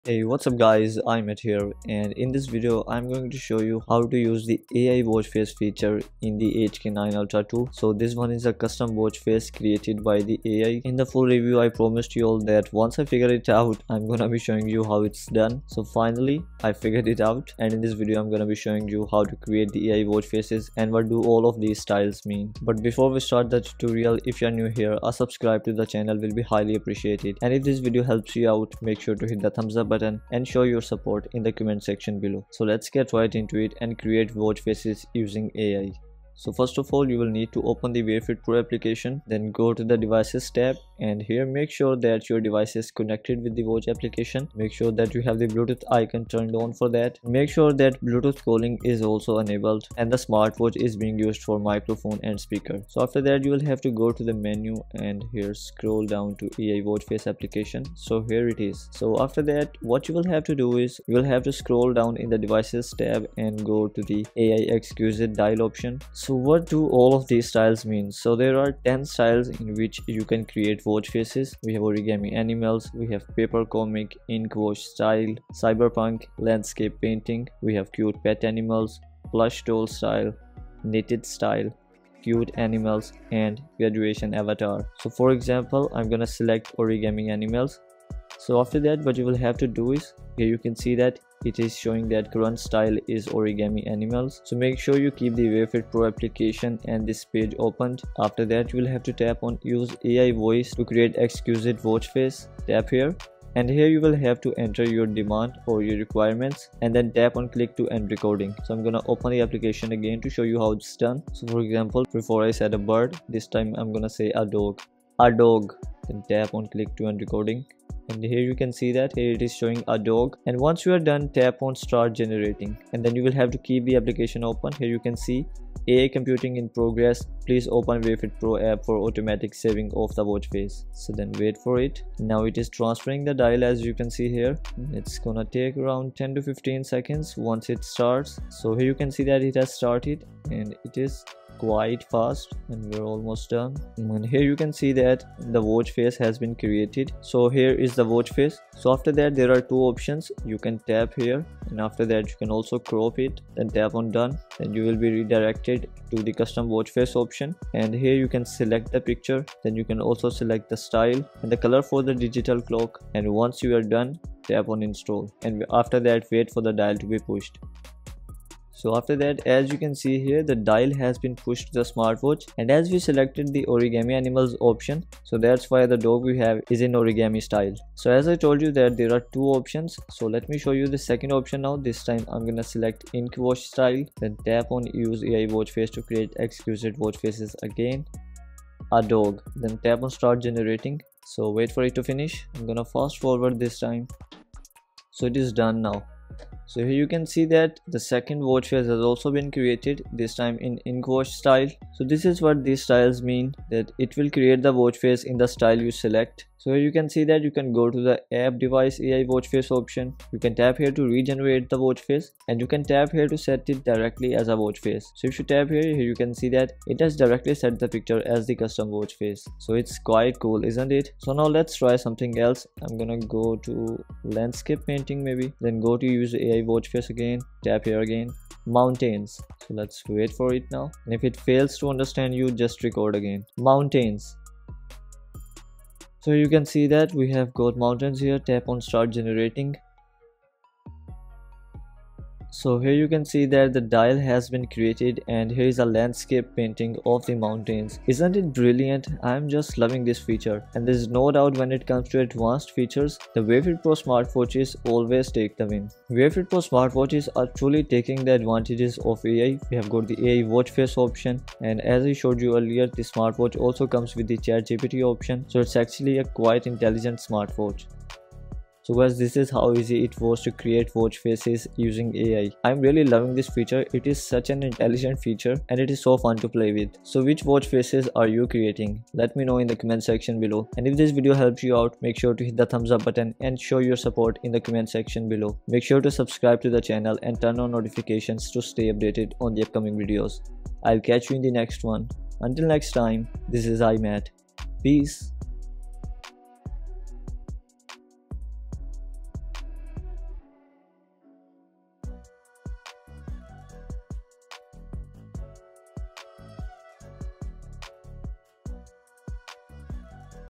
How to create Ai Watch Faces in HK9 Ultra 2/ HK9 Pro+ - Most Intelligent Apple Watch CLONE!
Camera: CANON 550D / Samsung NX2000
Mic: Blue Yeti Condenser Microphone / MAONO PM471 USB Microphone
Video Editor: Final Cut Pro
Thumbnail Editor: Adobe Photoshop